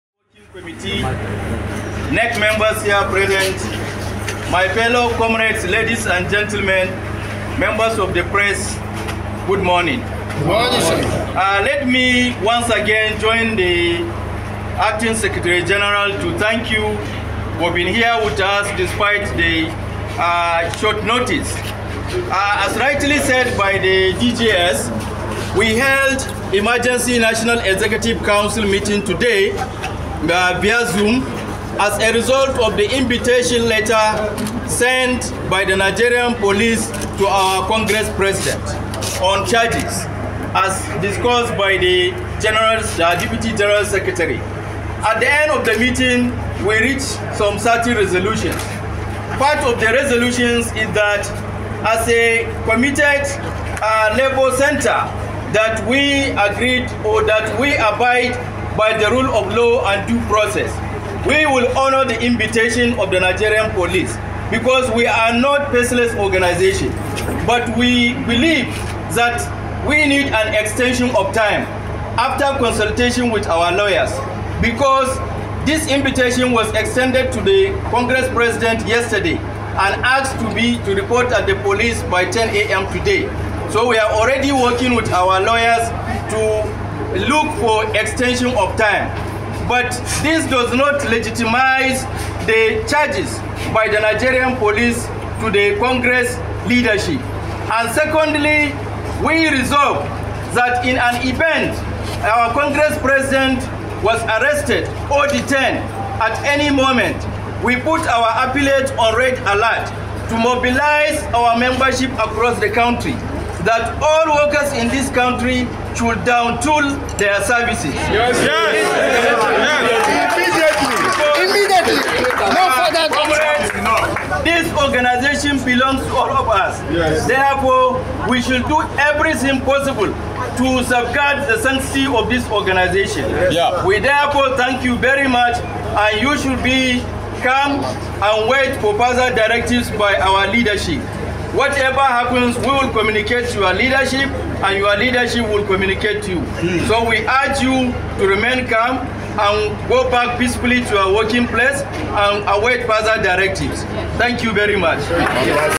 NLC Threatens Nationwide Strike if President Ajaero is Detained by Police
The Nigeria Labour Congress (NLC) has issued a stern warning, threatening to initiate a nationwide strike if its President, Comrade Joe Ajaero, is detained by the Nigeria Police. This declaration followed an emergency National Executive Council (NEC) meeting held on Tuesday morning at the NLC headquarters.

The meeting was convened to discuss the recent police invitation of Comrade Ajaero over allegations of terrorism financing. Addressing the press, NLC Deputy President, Comrade Ado Sani, urged the nation's workforce to remain on standby for further instructions, emphasizing that any action by the police contrary to their expectations would trigger a full-scale shutdown of labor activities across the country.

The NEC resolved that Comrade Ajaero should comply with the police invitation but noted that their legal adviser should seek an extension to allow more time for the NLC President to appear before the authorities. The union's stance underscores its readiness to defend its leadership and protect its members from what it perceives as undue harassment.